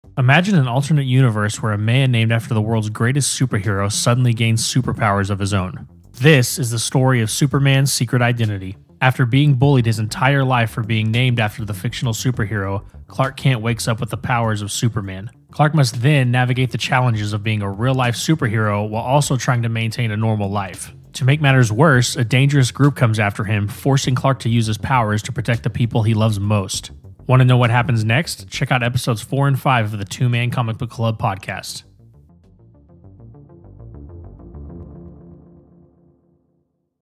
Which Guardians of the Galaxy character will James Gunn cast in his Superman film?
What if YOU woke up with SUPER POWERS?!

Listen to the 2Man Comic Book Club Podcast wherever you get your podcasts!